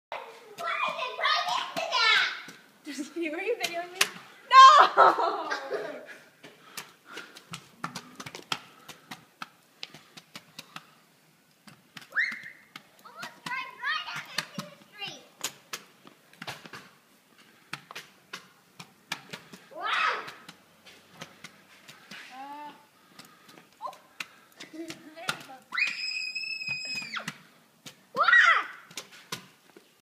Scooters
After weeks of snow, my daughters were so excited by the sunshine that they had to go ride their scooters.